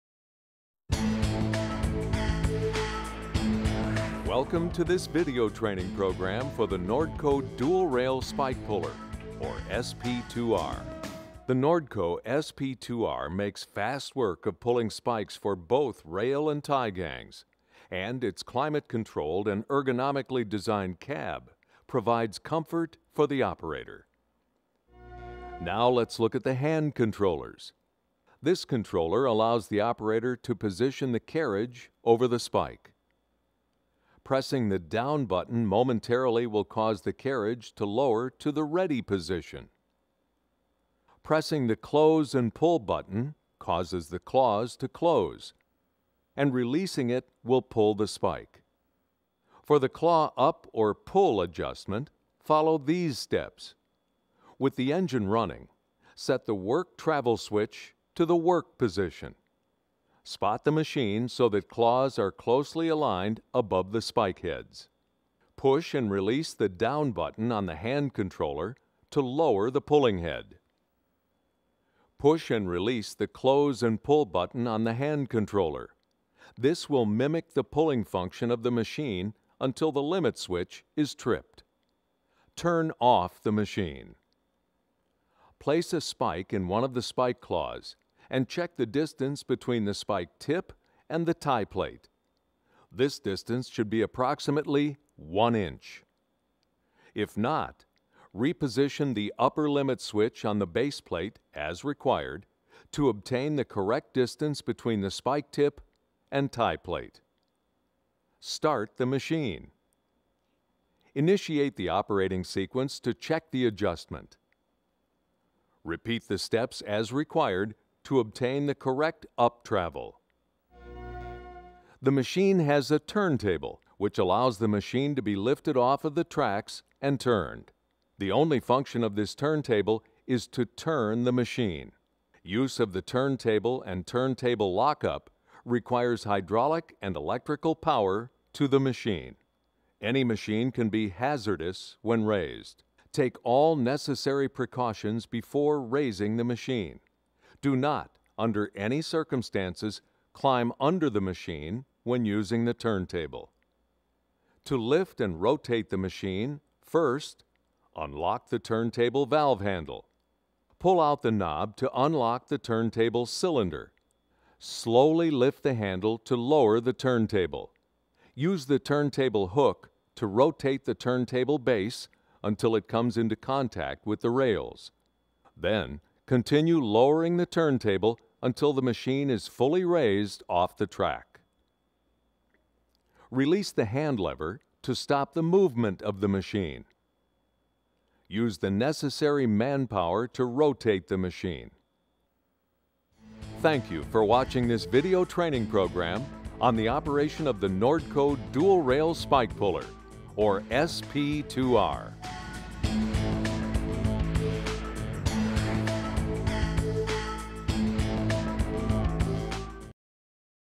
Railroad Construction Training Video
Training railroad crews across the vast and remote regions of North America demands clear, precise instruction. This video was created for NORDCO to provide hands-on, step-by-step training on how to safely and effectively operate their railroad repair equipment.

Whether on busy mainlines or remote tracks, this video ensures railroad professionals are fully prepared to maintain and repair infrastructure efficiently and safely.